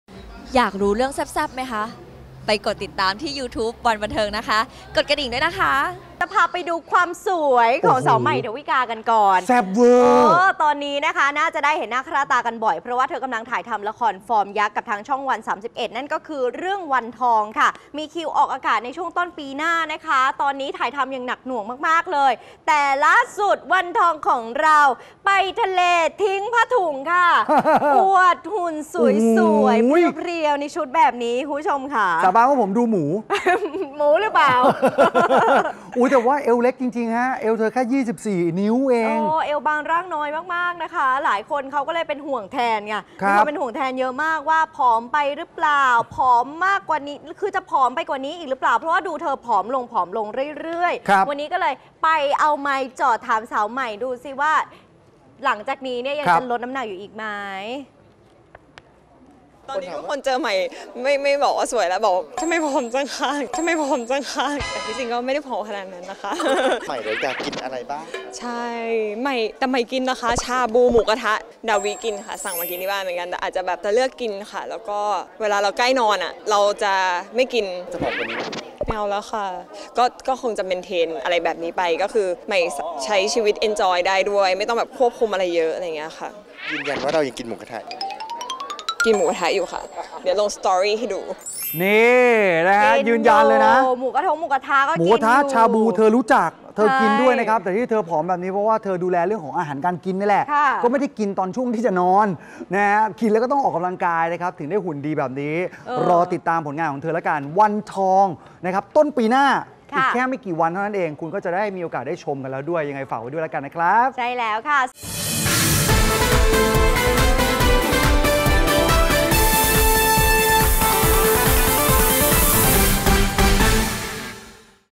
"ใหม่" กินทุกอย่าง แต่โดนทักผอมเกินไป | ประเด็นร้อน
สวยหุ่นเซี๊ย จนหลายคนทัก "ใหม่ ดาวิกา" รู้จักหมูกระทะไหม งานนี้เจ้าตัวลั่น! กินทุกอย่าง ไม่ได้ผอมขนาดนั้น